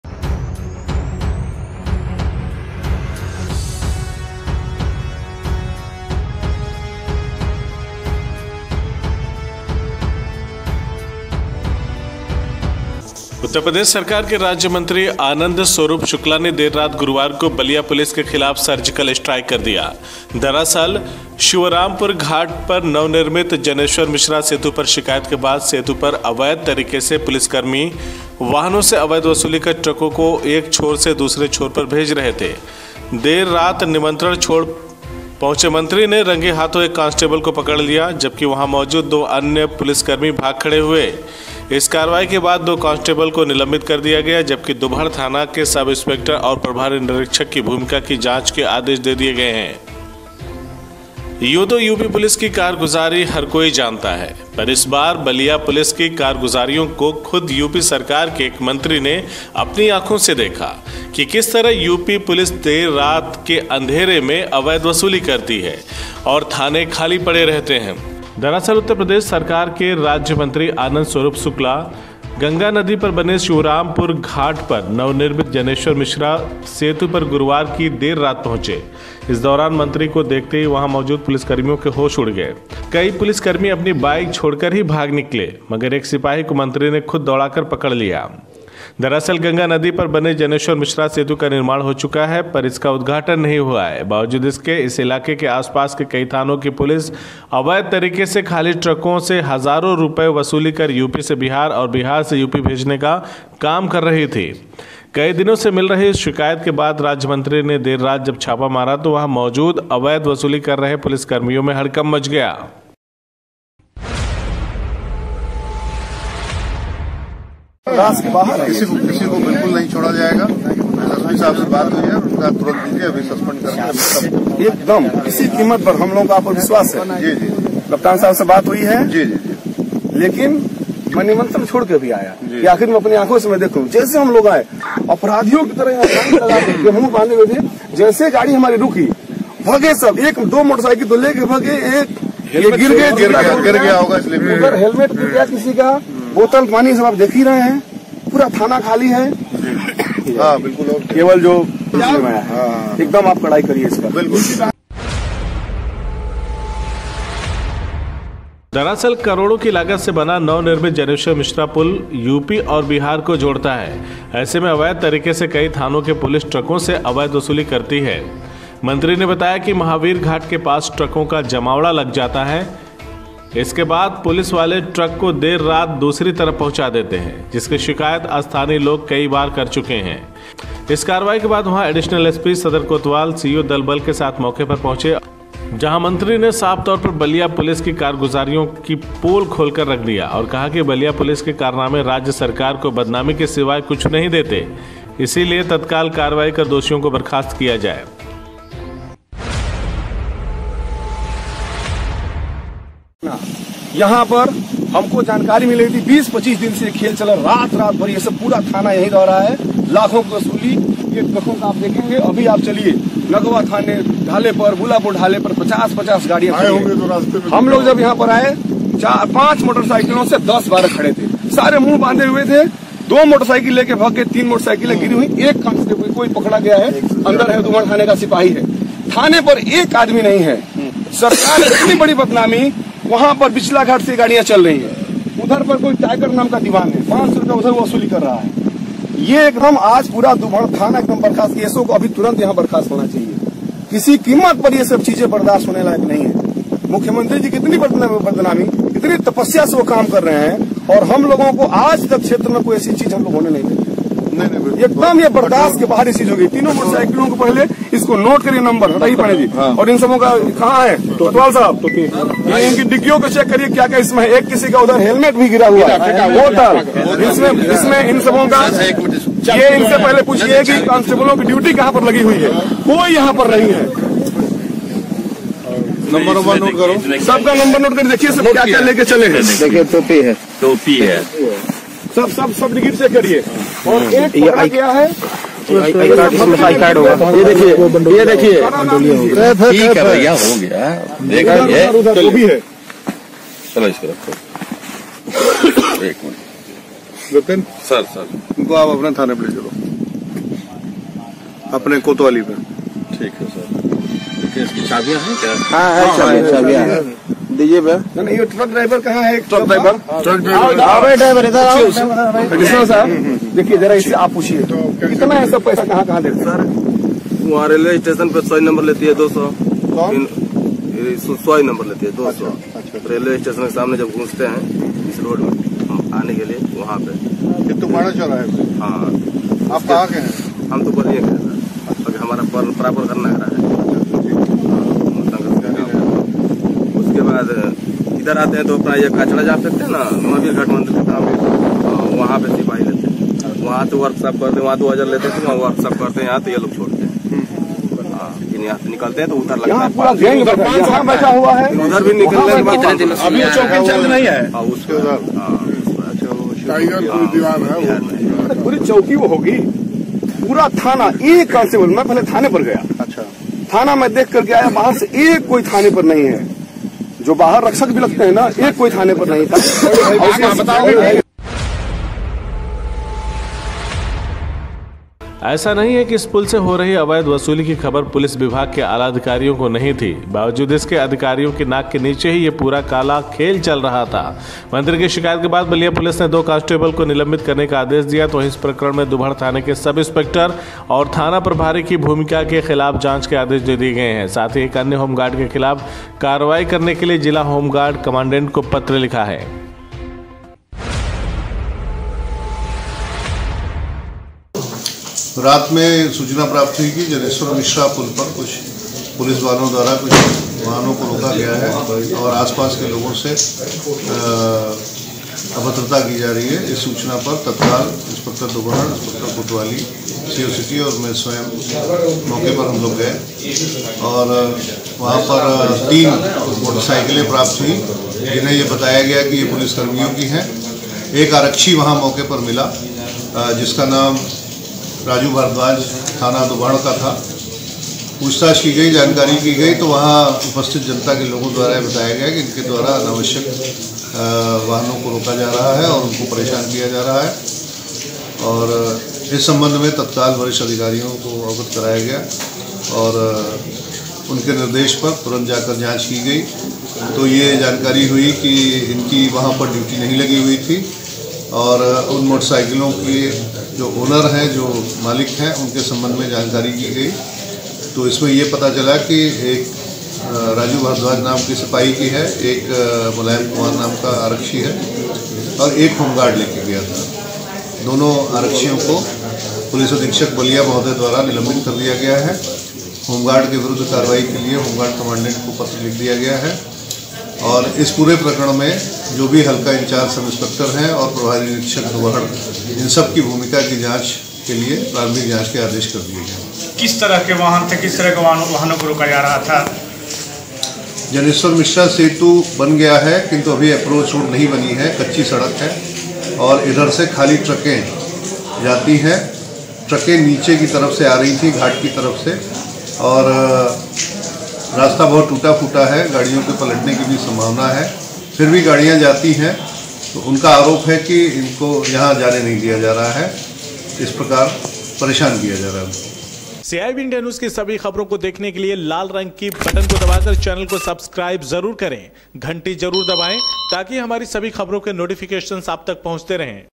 योगी के मंत्री का पुलिस के खिलाफ चला सर्जिकल स्ट्राइक..
उत्तर प्रदेश सरकार के राज्यमंत्री आनंद स्वरूप शुक्ला ने देर रात गुरुवार को बलिया पुलिस के खिलाफ सर्जिकल स्ट्राइक कर दिया| दरअसल शिवरामपुर घाट पर नवनिर्मित जनेश्वर मिश्रा सेतु पर शिकायत के बाद सेतु पर अवैध तरीके से पुलिसकर्मी वाहनों से अवैध वसूली कर ट्रकों को एक छोर से दूसरे छोर पर भेज रहे थे| देर रात निमंत्रण छोड़ पहुंचे मंत्री ने रंगे हाथ एक कॉन्स्टेबल को पकड़ लिया जबकि वहां मौजूद दो अन्य पुलिसकर्मी भाग खड़े हुए| इस कार्यवाही के बाद दो कांस्टेबल को निलम्बित कर दिया गया जबकि दुबहड़ थाना के सब इंस्पेक्टर और प्रभारी निरीक्षक की भूमिका की जाँच के आदेश दे दिए गए है|